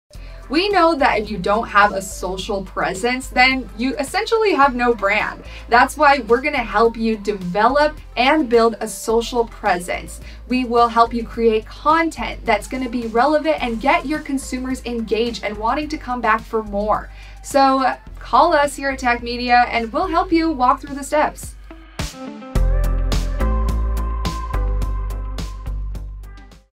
Social Presence Content Creation | Tack Media Digital Agency
Desc: If you don’t have a social presence then you basically don’t have any brand. Tack Media is a full service digital marketing firm and a digital transformation agency based in Los Angeles and will create content that will be relevant and get your consumers engaged and keep them coming back for more.